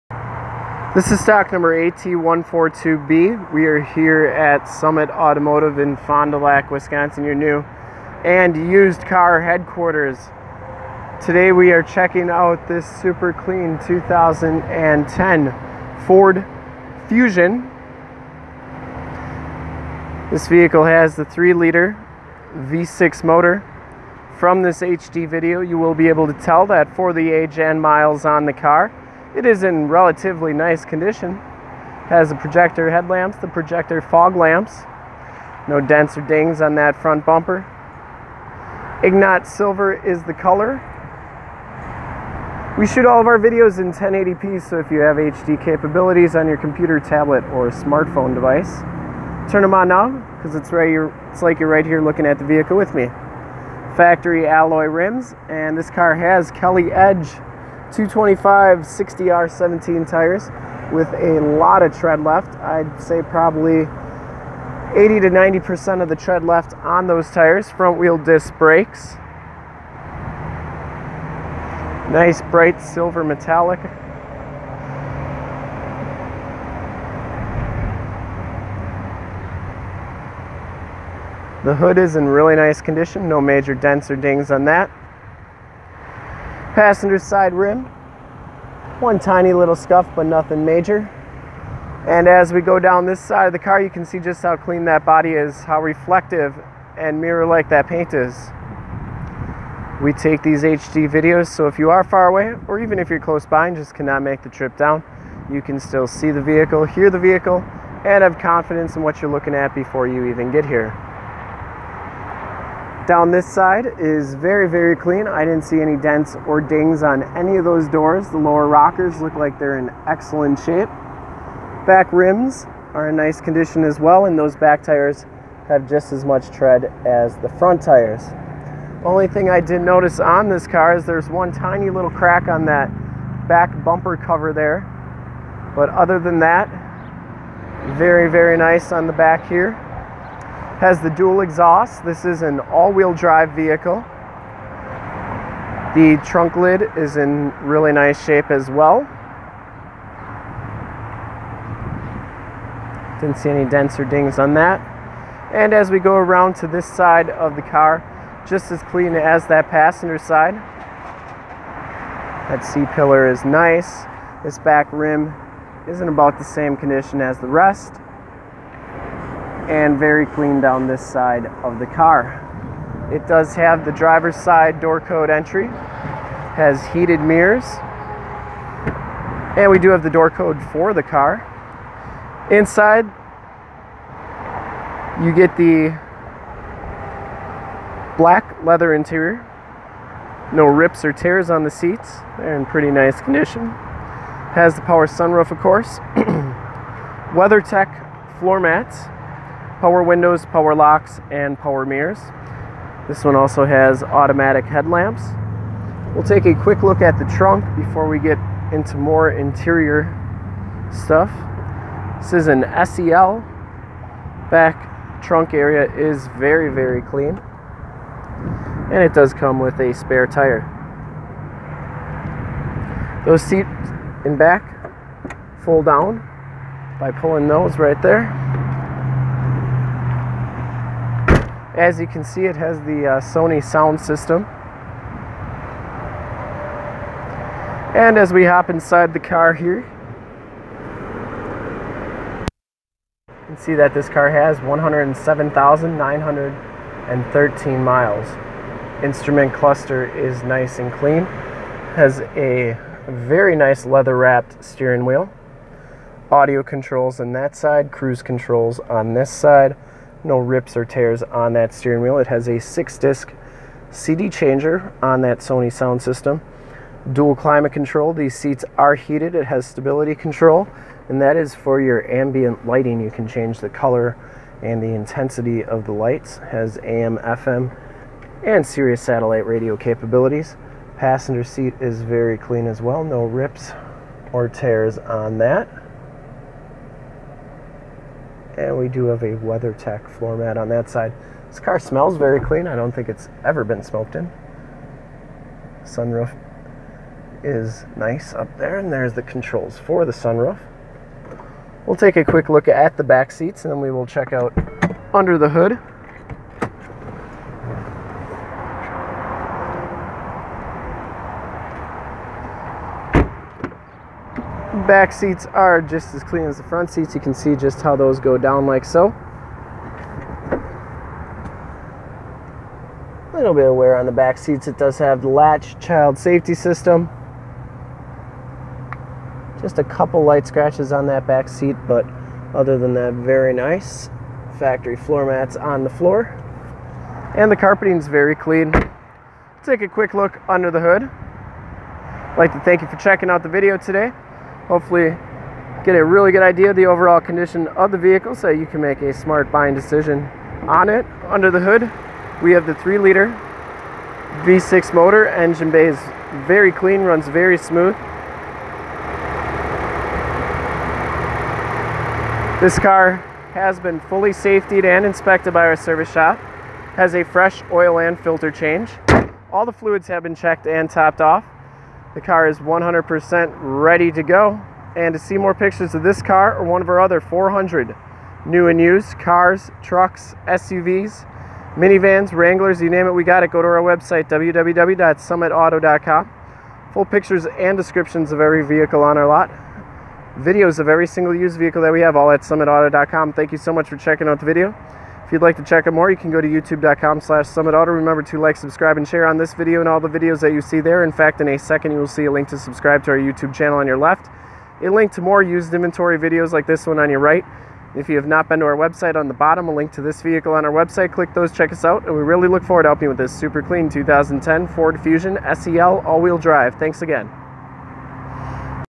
2010 FORD FUSION SEL AWD MOON INGOT SILVER WALKAROUND SOLD! 8T142B
STOCK: 8T142B
PRICE: $8,799
MILES: 107,857
MAKE: FORD
MODEL: FUSION
VIN: 3FAHP0CG8AR263108
LOCATION: FOND DU LAC OSHKOSH WISCONSIN, 54937 TRUCKS ON 41

CLEAN TITLE HISTORY! 3.0 Liter V6 DOHC Engine with E85 Capabilities, 240 Horsepower, Four Door Sedan, SEL Package, Automatic Transmission Center Console Shifter, Front Wheel Drive FWD, Power Sunroof Moonroof Sun Roof Moon Roof, Dual Rear Exhaust, Dual Power Heated Seats, Non Smoker, Black Ebony Leather Seats, Bucket Seats, Heated Power Mirrors, Advancetrac with Electronic Stability Traction Contol, Michelin Primacy 225/50 R17 Tires, Factory Alloy Rims Premium Wheels, Four Wheel Disc Brakes, Fog Lights, Projector Lamp Headlights, Securicode Driver Side Doorcode Keyless Entry, Easy Fuel Capless Fuel Fill, Chrome Tipped Exhaust, Chrome Trimmed Grill, AM / FM Radio Tuner, Sirius/XM Satellite Radio Capabilities Sirius / XM, 6 Disc in-dash CD Changer CD Player, Sony Premium Audio Sound System, Microsoft SYNC System with Bluetooth, Hands-Free Audio System Blue Tooth, Auxiliary MP3 Jack Portable Audio Connection, USB Jack Portable Audio Connection, Keyless Entry System, Rear Window Defroster, Adjustable Height Seatbelts, Driver and Passenger Front Air Bags, L.A.T.C.H. Child Safety System, Side Curtain Air Bags SRS Safety Restraint System, Multi-Function Steering Wheel Controls, Compass, Outside Temperature Display and Mileage Display, Dual Multi-Zone Climate Control , Fold Down Rear Seats, Weathertech Floormats, Ambient Color Change Lighting, Air Conditioning AC, Cruise Control, Power Locks, Power Windows, Automatic Headlights Autolamp, Tilt/Telescope Steering Wheel, Silver, Clean Autocheck! Very very clean inside and out! This is one of the sharpest 2010 Ford Fusion cars we have ever had on our lot! Make your move before this super clean awd is gone!

STOCK: 8T142B
PRICE: $8,799
MILES: 107,857
MAKE: FORD
MODEL: FUSION
VIN: 3FAHP0CG8AR263108